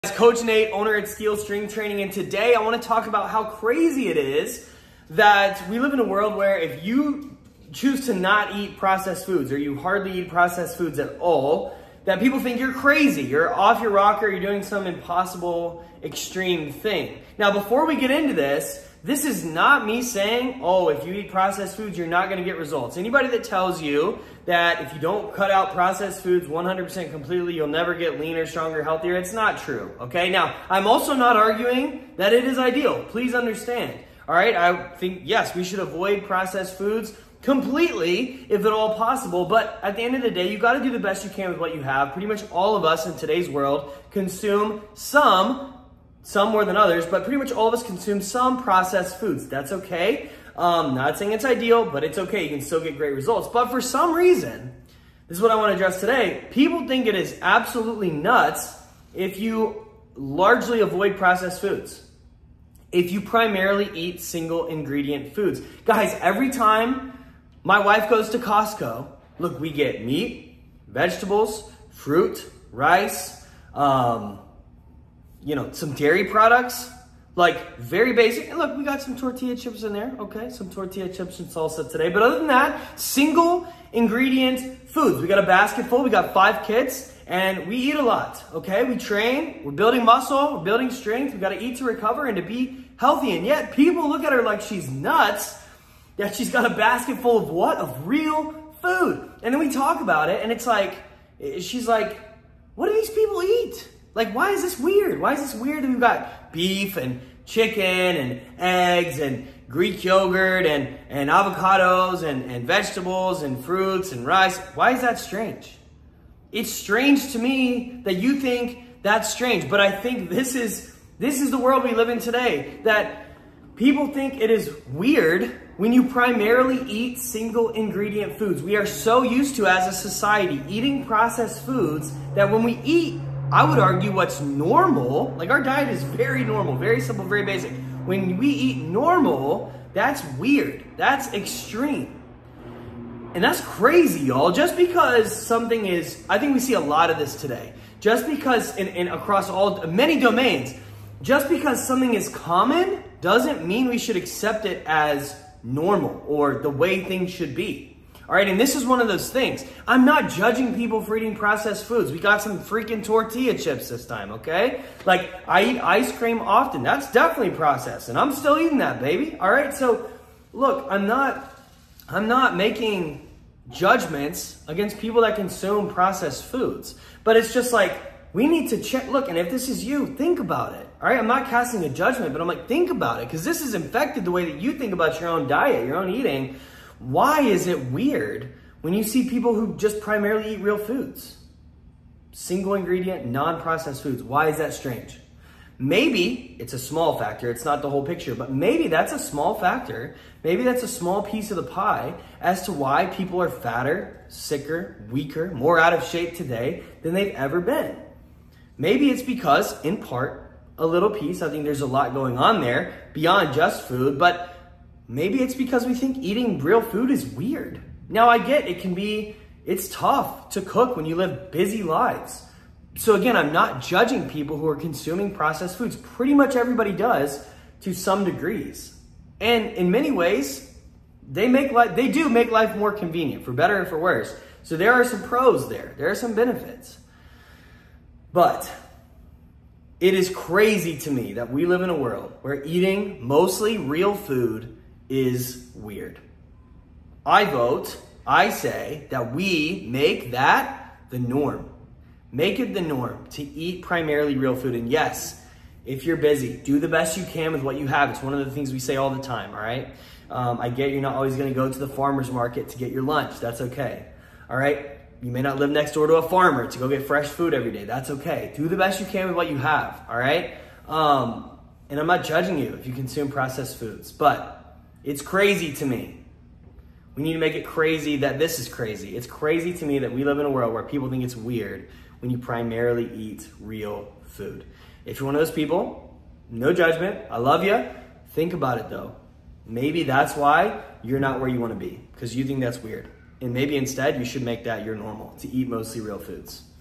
Why Is Not Eating Processed Foods Considered Strange
The only strange concept here is the fact people think it's weird to eat single-ingredient foods! We live in a society where processed foods are the norm and that is NOT how it should be!

You’d still get results eating them, but you will get even greater results, have better health benefits, and feel better all around. Let’s change the fad and start looking at processed foods strangely instead!